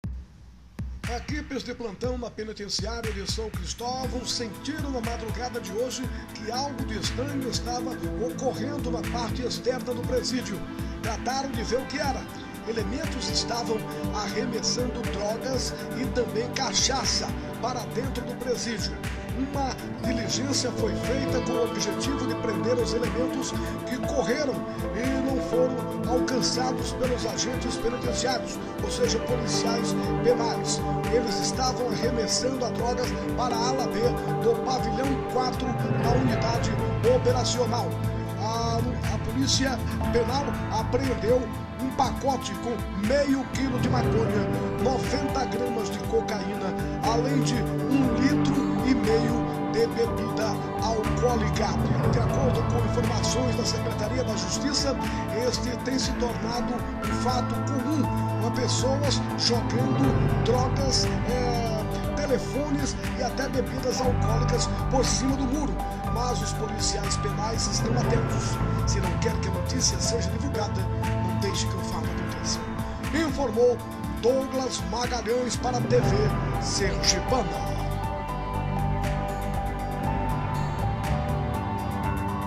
Policia flagra arremeso de maconha e cachaça para dentro da Penitenciária de São Cristovão
Equipes de plantão do Copemcan frustram tentativa de arremesso de drogas para a unidade prisional;

Todo o material foi apreendido na madrugada desta sexta-feira, 29 Uma tentativa de arremesso de drogas para o Complexo Penitenciário Dr. Manoel Carvalho Neto (Copemcan), que fica em São Cristóvão, foi frustrada pelos plantonistas da unidade prisional. A ação ocorreu durante a madrugada desta sexta-feira, 29. De acordo com a direção da unidade, a tentativa […]
Uma tentativa de arremesso de drogas para o Complexo Penitenciário Dr. Manoel Carvalho Neto (Copemcan), que fica em São Cristóvão, foi frustrada pelos plantonistas da unidade prisional. A ação ocorreu durante a madrugada desta sexta-feira, 29.
De acordo com a direção da unidade, a tentativa de arremesso ocorreu para a Ala B do Pavilhão 4 da unidade prisional. As equipes de plantão perceberam a movimentação ao redor do presídio.

Foram feitas diligências mas os suspeitos da ação não foram alcançados. No entanto, todo o material foi apreendido. Nos pacotes, foram encontrados 500g de maconha, 90g de cocaína, além de 1,5 litro de bebida alcoólica.